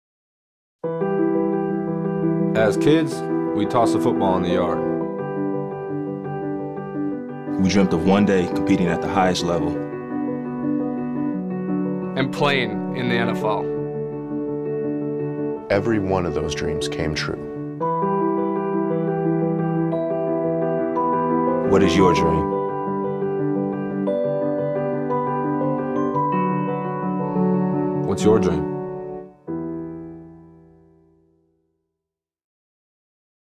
NX Level - What Is Your Dream?
NX Level Athletics, Milwaukee, WI
Owner - Brad Arnett

Part of a series of spots directed by David Michael Cohen for NX Level featuring, J.J. Watt, DeAndre Levy, Kevin Zeitler, Nick Hayden, Bill Nagy and Brad Arnett.